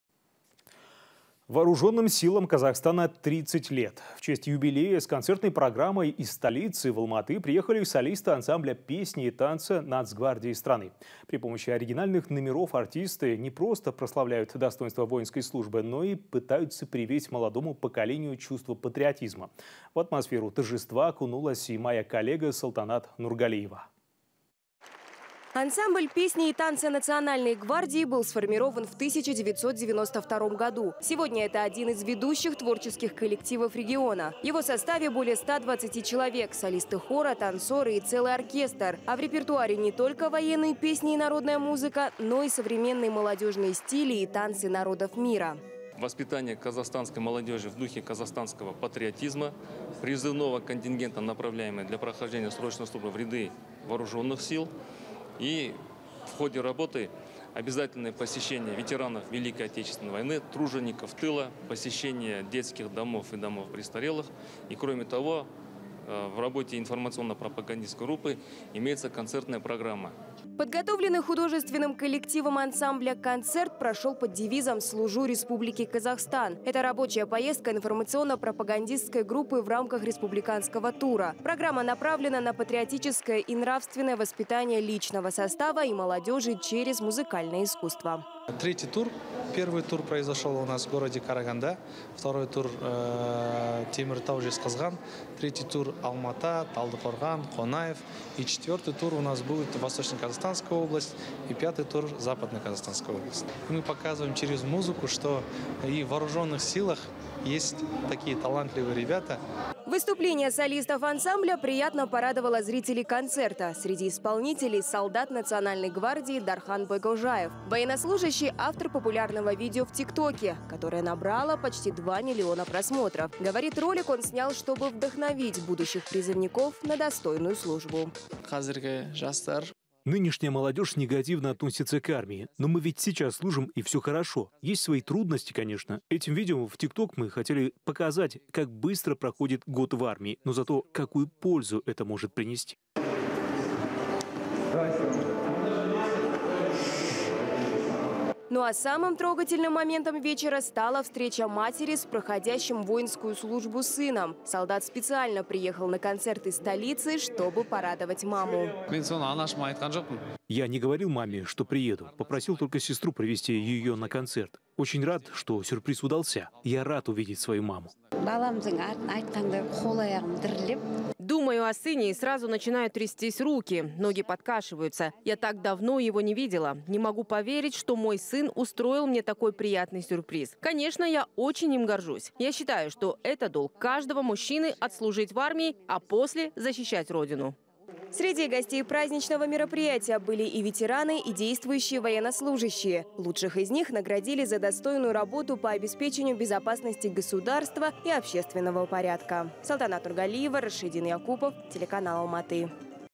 Вооруженным силам Казахстана - 30 лет: в Алматы прошел концерт ансамбля Нацгвардии
Вооруженным силам Казахстана - 30 лет!  В честь юбилея с концертной программой из столицы в Алматы приехали солисты ансамбля песни и танца Нацгвардии страны. При помощи оригинальных номеров артисты не просто прославляют достоинства воинской службы, но и пытаются привить молодому поколению чувство патриотизма, передает корреспондент Almaty.tv.